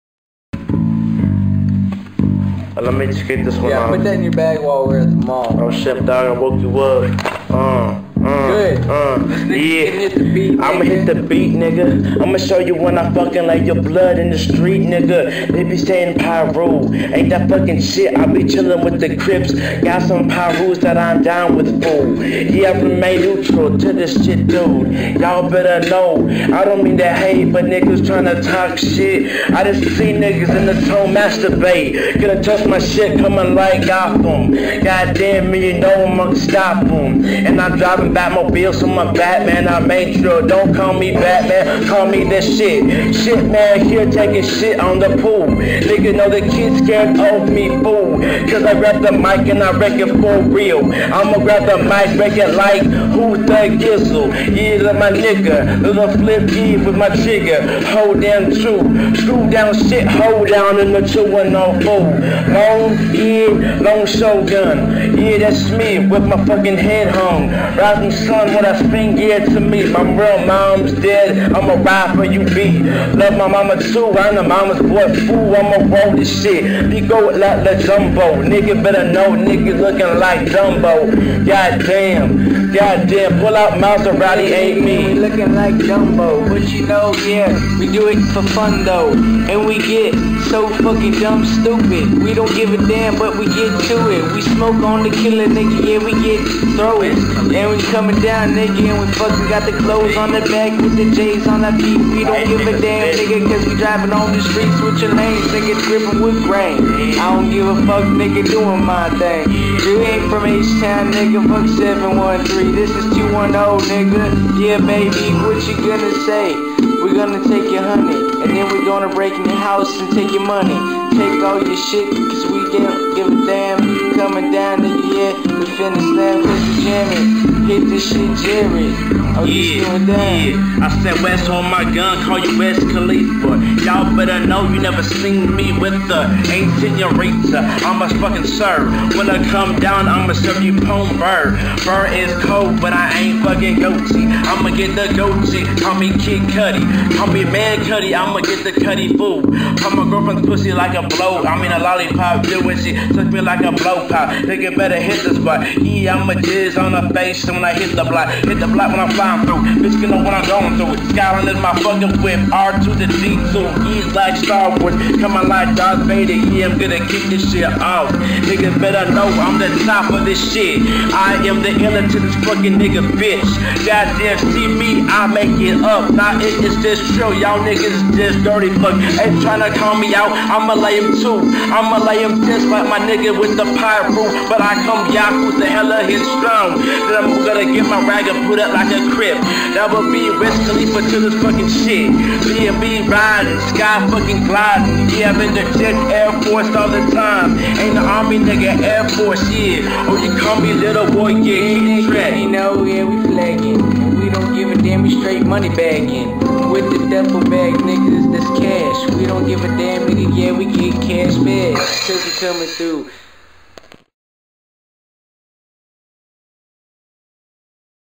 Bakkjadez ft WES aka Wes khalifa- freestyle.wmv
I was in my x home girls mustang an me an my igga wes aka wes khalifa freestyle this sht lol